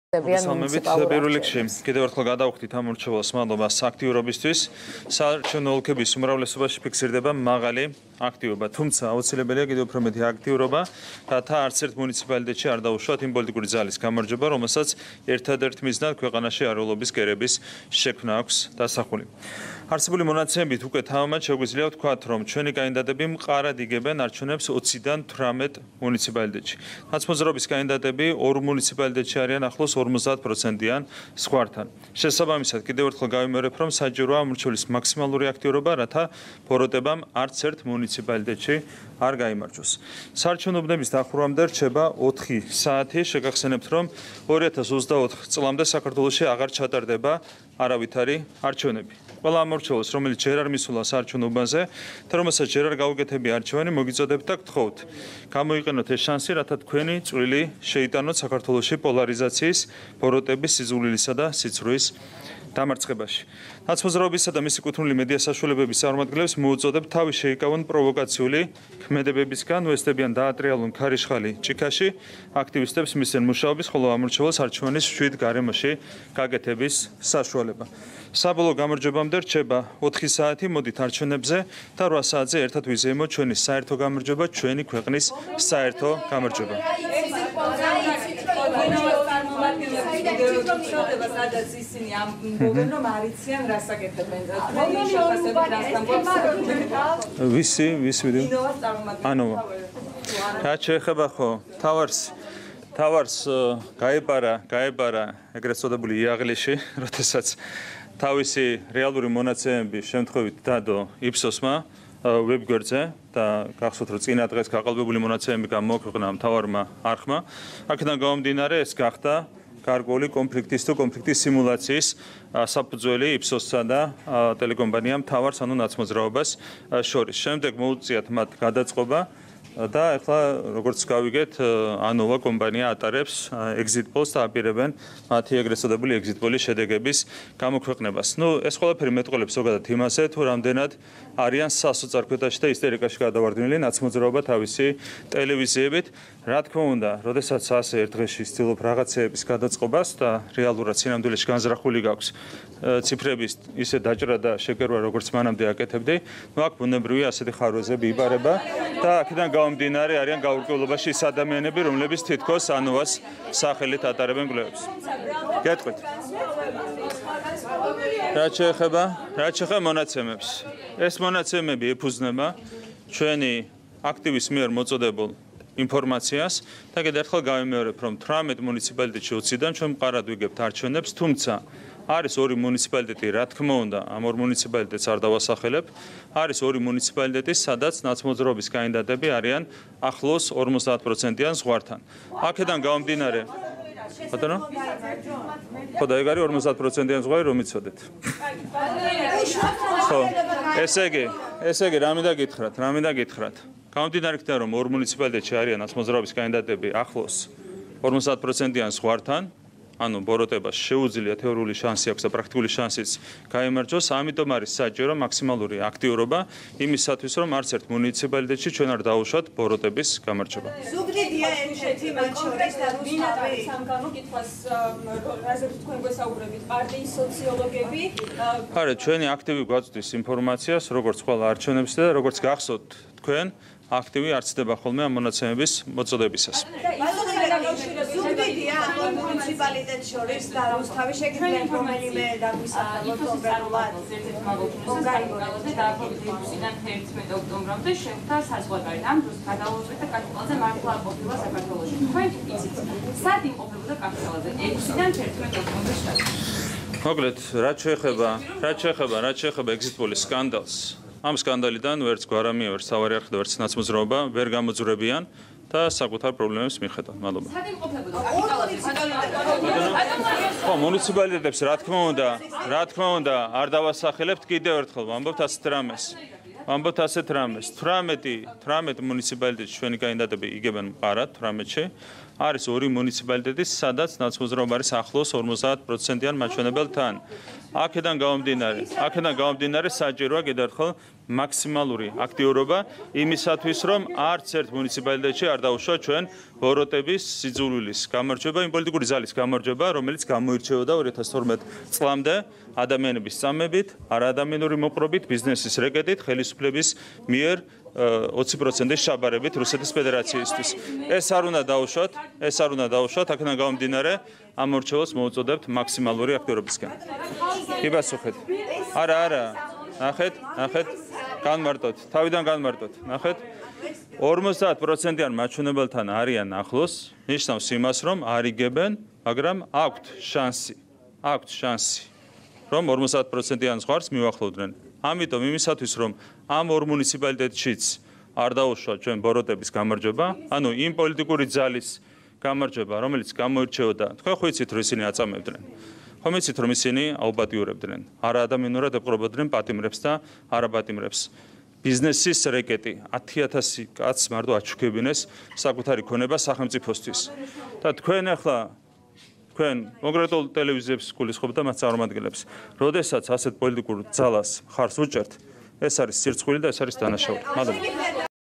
ირაკლი კობახიძის ბრიფინგი
პირველ რიგში, კიდევ ერთხელ გადავუხდით ამომრჩეველს მადლობას აქტიურობისთვის. საარჩევნო ოლქების უმრავლესობაში ფიქსირდება მაღალი აქტივობა. თუმცა, აუცილებელია კიდევ უფრო მეტი აქტიურობა, რათა არცერთ მუნიციპალიტეტში არ დავუშვათ იმ პოლიტიკური ძალის გამარჯვება, რომელსაც ერთადერთ მიზნად ქვეყანაში არეულობის კერების შექმნა აქვს დასახული.
არსებული მონაცემებით, უკვე თამამად შეგვიძლია ვთქვათ, რომ ჩვენი კანდიდატები მყარად იგებენ არჩევნებს 20-დან 18 მუნიციპალიტეტში. ნაცმოძრაობის კანდიდატები ორ მუნიციპალიტეტში არიან ახლოს 50-პროცენტიან ზღვართან. შესაბამისად, კიდევ ერთხელ გავიმეორებ, რომ საჭიროა ამომრჩევლის მაქსიმალური აქტიურობა, რათა ბოროტებამ არცერთ მუნიციპალიტეტში არ გაიმარჯვოს.
საარჩევნო უბნების დახურვამდე რჩება 4 საათი. შეგახსენებთ, რომ 2024 წლამდე საქართველოში აღარ ჩატარდება არავითარი არჩევნები. ყველა ამომრჩეველს, რომელიც ჯერ არ მისულა საარჩევნო უბანზე და რომელსაც ჯერ არ გაუკეთებია არჩევანი, მოგიწოდებთ და გთხოვთ, გამოიყენოთ ეს შანსი, რათა თქვენი წვლილი შეიტანოთ საქართველოში პოლარიზაციის, ბოროტების, სიძულვილისა და სიცრუის დამარცხებაში.
ნაცმოძრაობისა და მისი კუთვნილი მედიასაშუალებების წარმომადგენლებს მოვუწოდებთ, თავი შეიკავონ პროვოკაციული ქმედებებისგან, ნე ეცდებიან დაატრიალონ ქარიშხალი ჭიქაში, აქტივისტებს მისცენ მუშაობის, ხოლო ამომრჩეველს არჩევანის მშვიდ გარემოში გაკეთების საშუალება.
საბოლოო გამარჯვებამდე რჩება 4 საათი. მოდით არჩევნებზე და 8 საათზე ერთად ვიზეიმოთ ჩვენი საერთო გამარჯვება, ჩვენი ქვეყნის საერთო გამარჯვება.